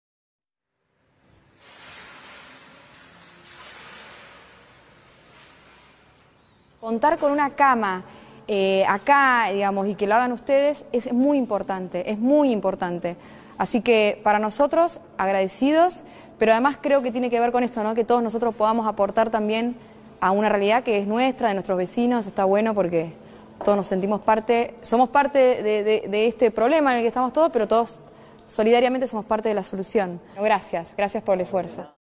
Astilleros Rìo Santiago.
🎦❤  Un orgullo.
Este trabajo es un agradecimiento a la sociedad, que nos banco en nuestra lucha.
Los trabajadores recibieron a Luana Volnovich, directora del PAMI.
La solidaridad es un bien de los seres humanos imprescindible para su subsistencia.